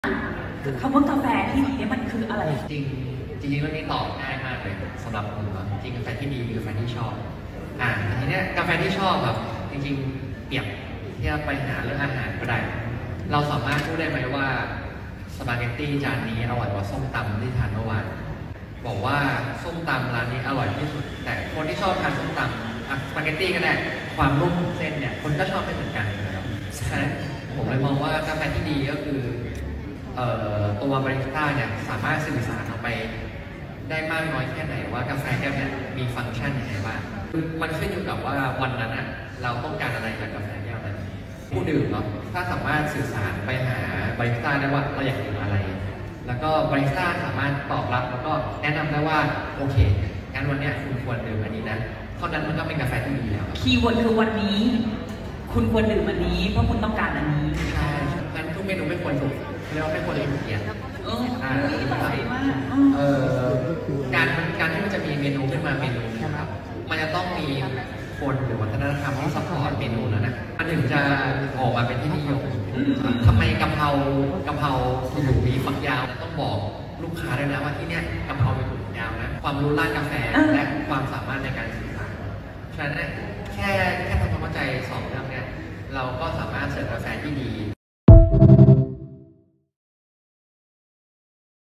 EP02 กาแฟดี ของแชมป์บาริสต้า เป็นยังไง?
worldcoffeeevent EP02 กาแฟดี ของ คุณ ฟิวเจอร์ กิตติภพ เอ่งฉ้วน แชมป์บาริสต้าคนล่าสุดของไทย เป็นยังไง

ขอบคุณร้าน davincafethailand  นะคะ